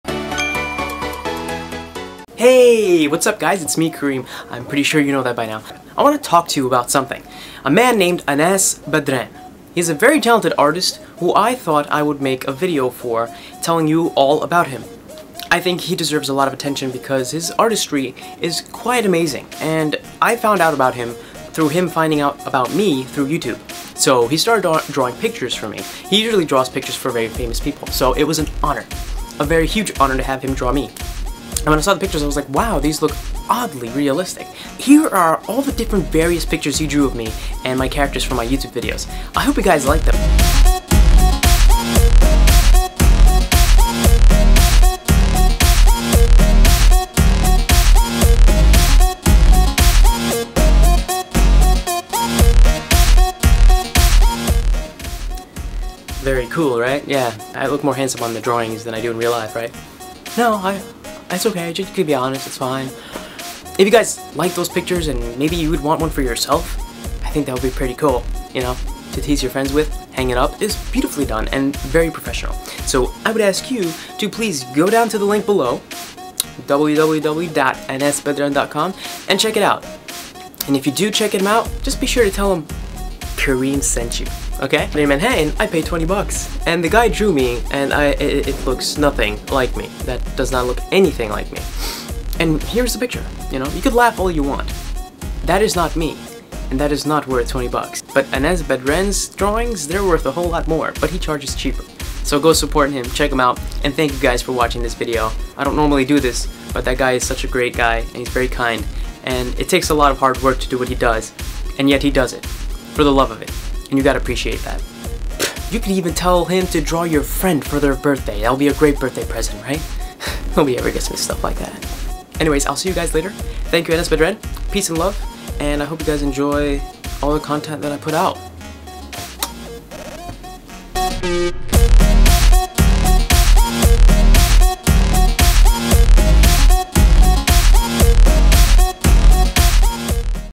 Anas Badran the Talented Artist
My Wish List Please watch my New Prank Video!

Just wanted to give some recognition to this kind and talented guy Anas Badran.